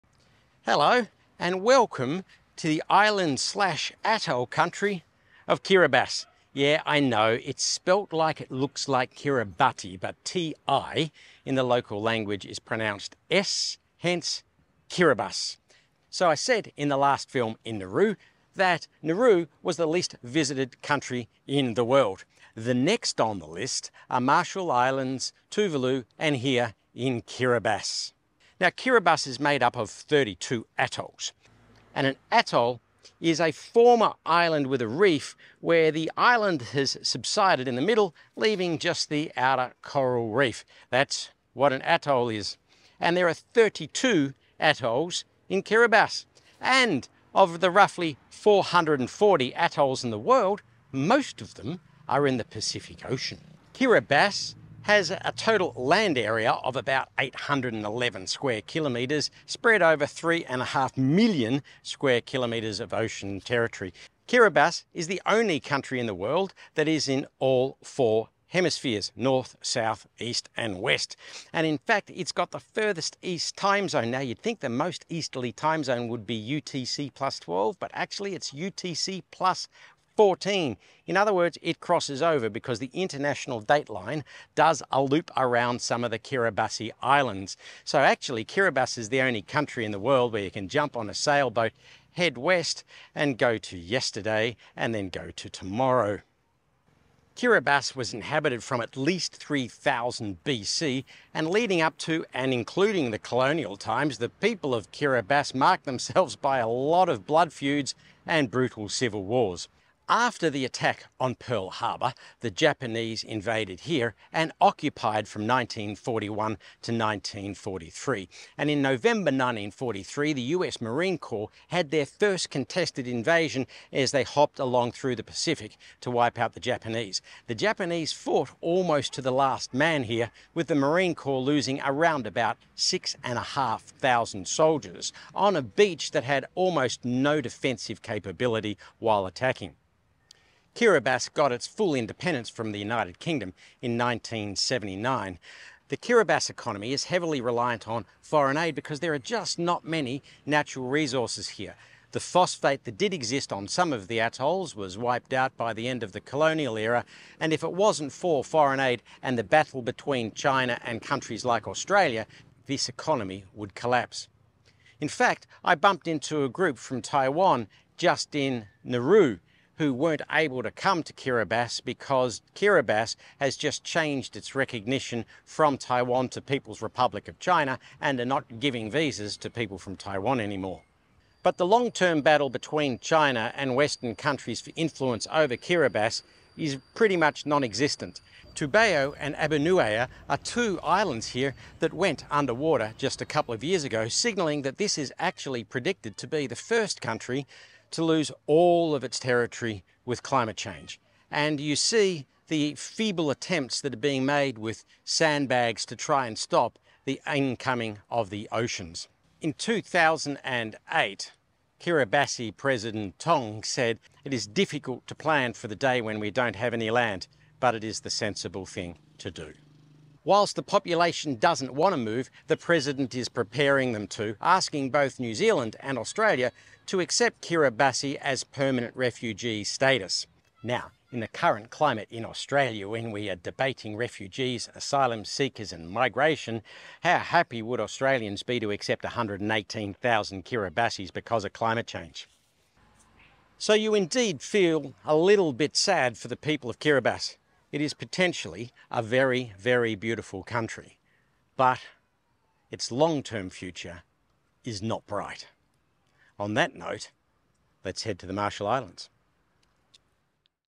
179 Kiribati: One of four least visited countries.
Kiribati (pronounced Kiribass as the 'ti' is an 's' here).

The guns are Japanese from WW2. Storming the beaches here must have been a nightmare for the US Marines as there is absolutely no cover.